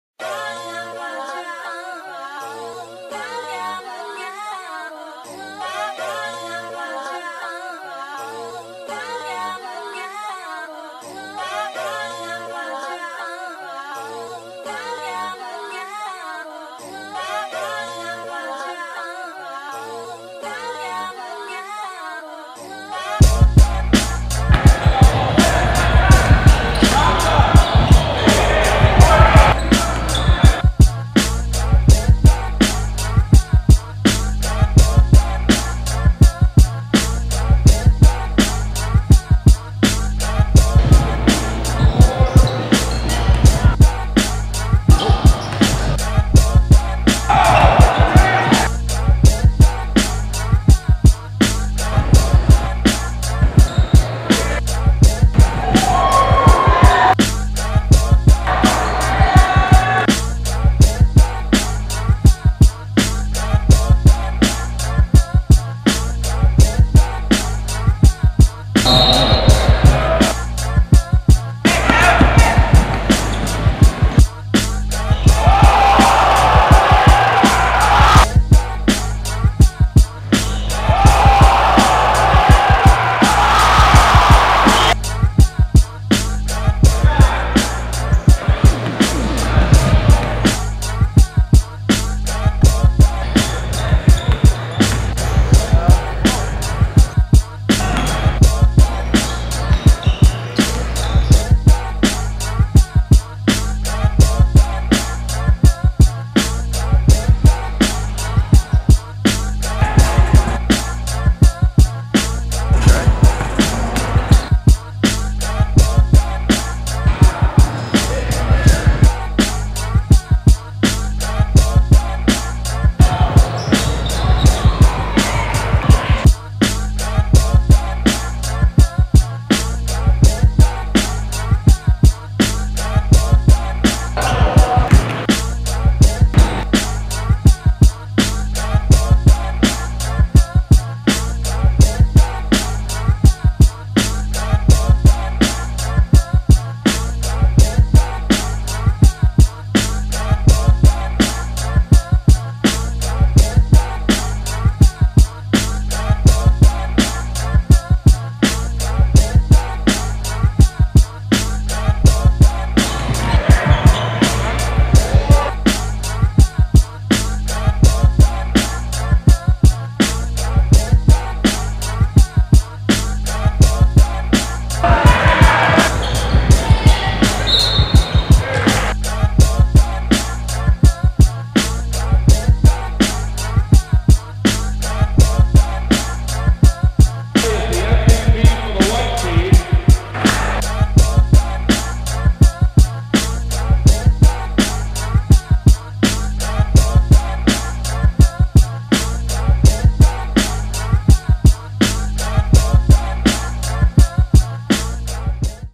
8Eye Future Classic - Class of 2019 (7th Grade)
8Eye Future Classic for 7th Graders Official HighLight Mix!
The Future was on display in Philly, in a game filled with talent from the NY, NJ, Delaware and Philadelphia region.